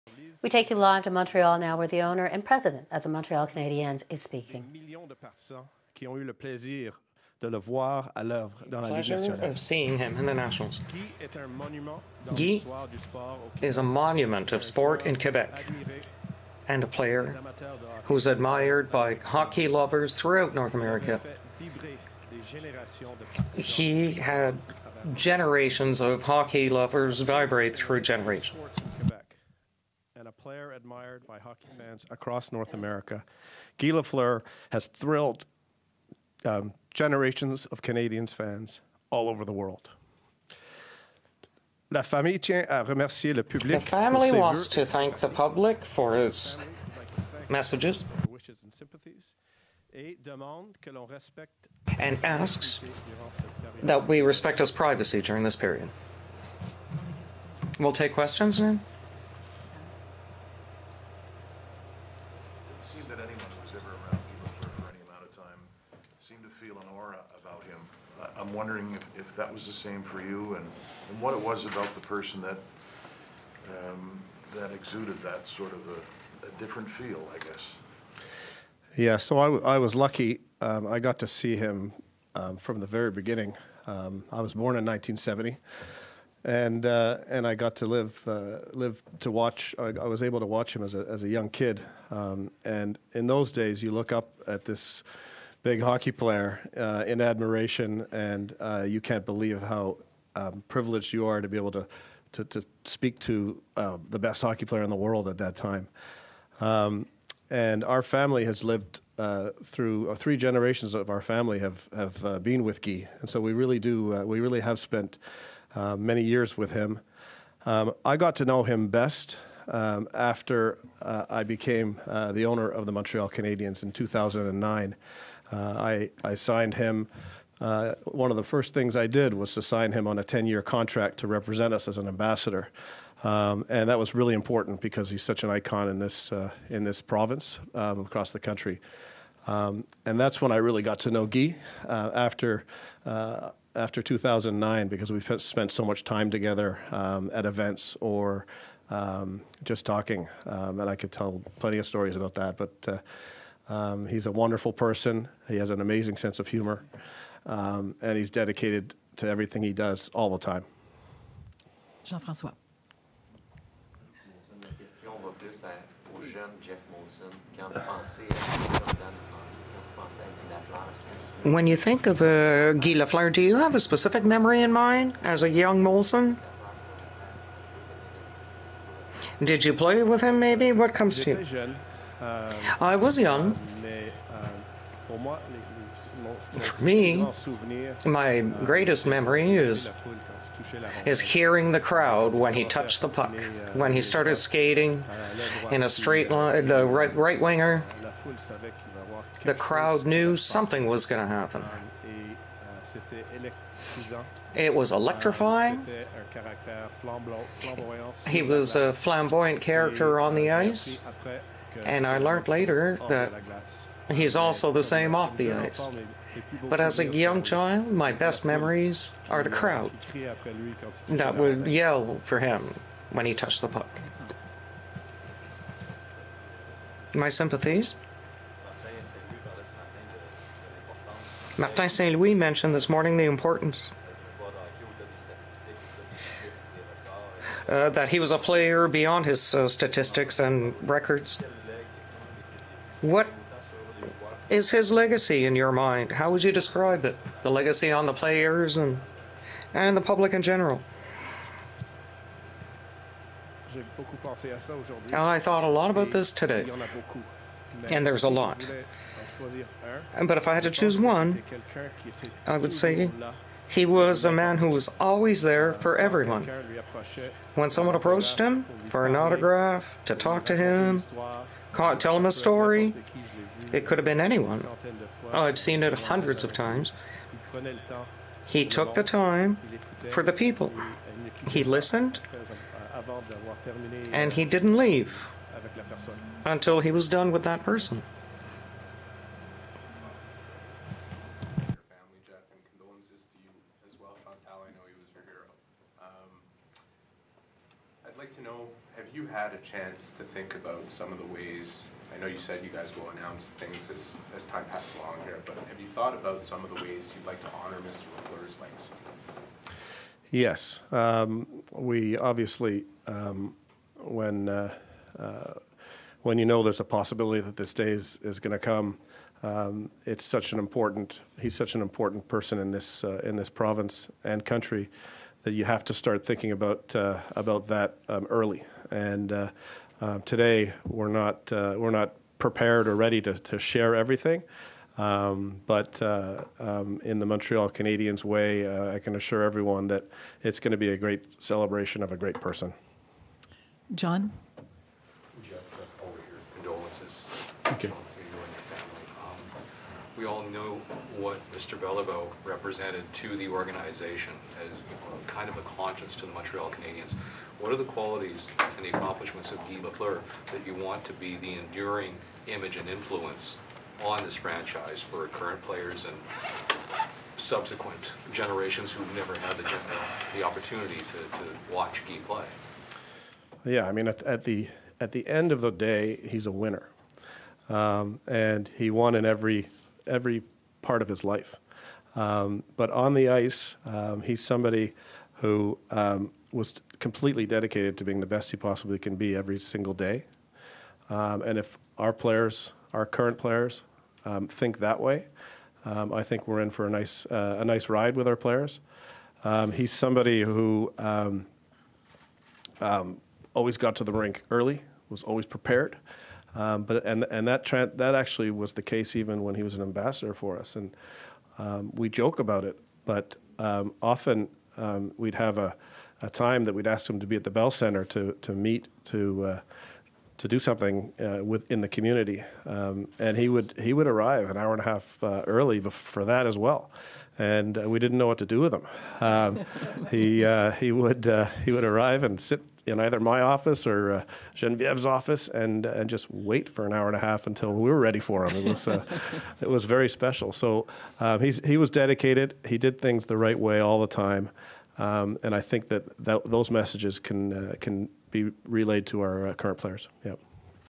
'He's an icon': Montreal Canadiens owner Geoff Molson shares his thoughts on Guy Lafleur
Montreal Canadiens owner Geoff Molson shared his thoughts following the death of hockey Hall of Famer Guy Lafleur.